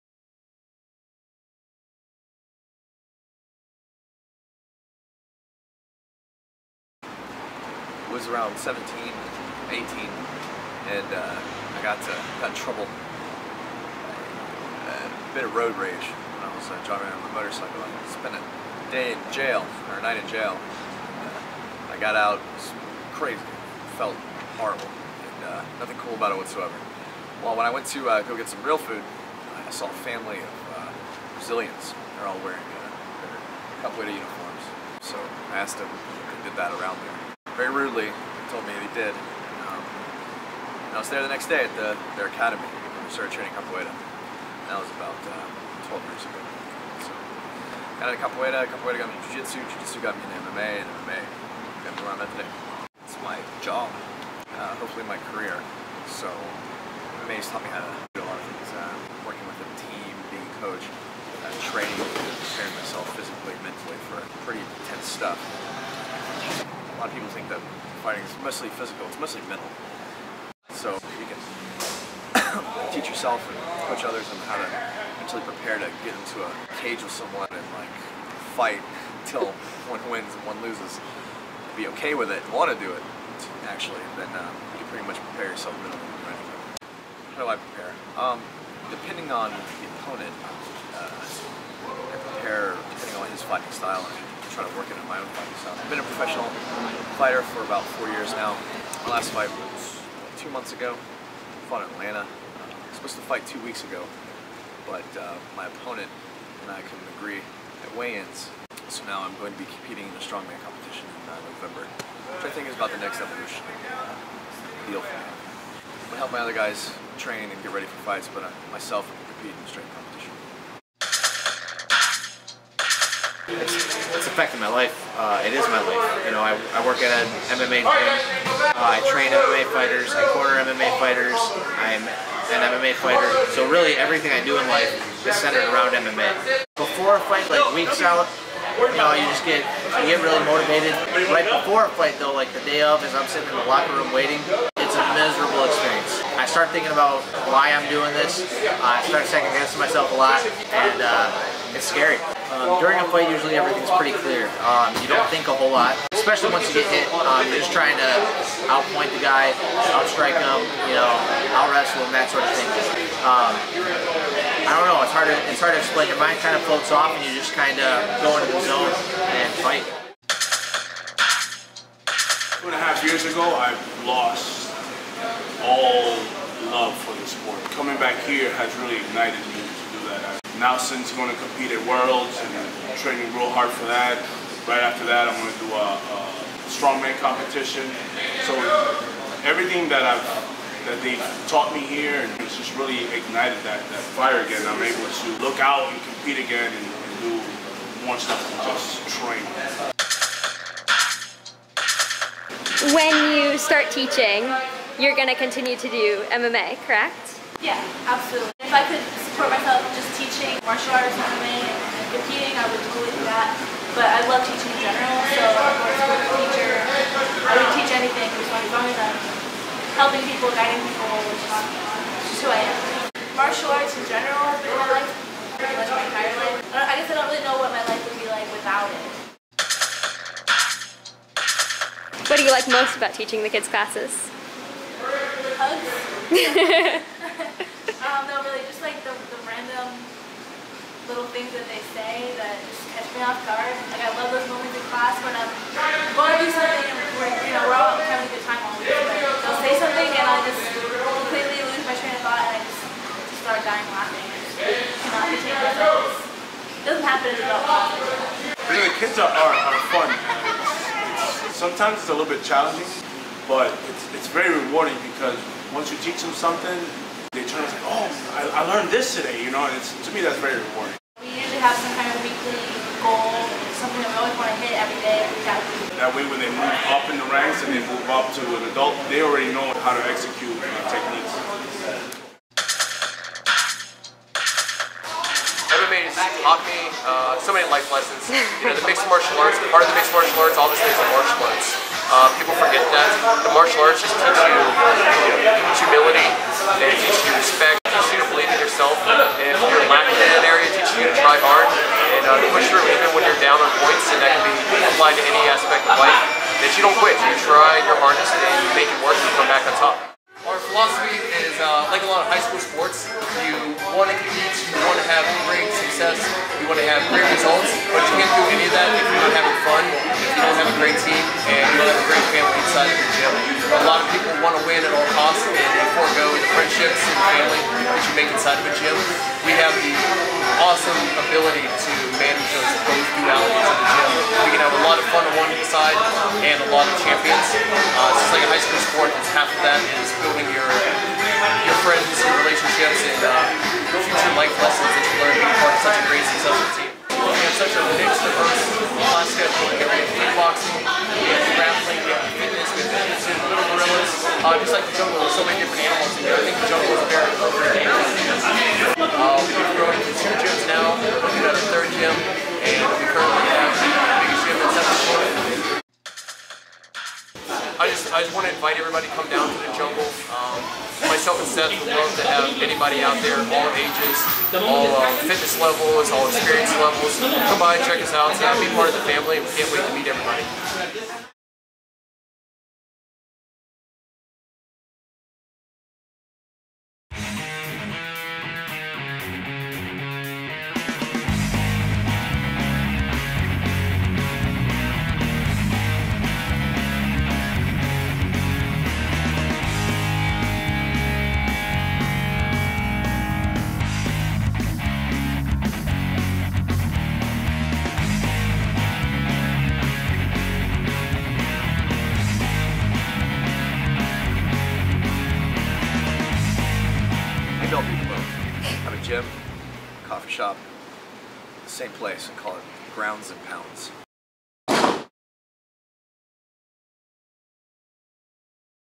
The Jungle MMA & Fitness Documentary
A documentary about The Jungle MMA and Fitness in downtown Orlando featuring interviews with Mike Lee, Aaron Conway, Felicia Spencer, John Morehouse, and Kevin Abrante.

Directed, Edited, Filmed, and Produced by Sarah Pender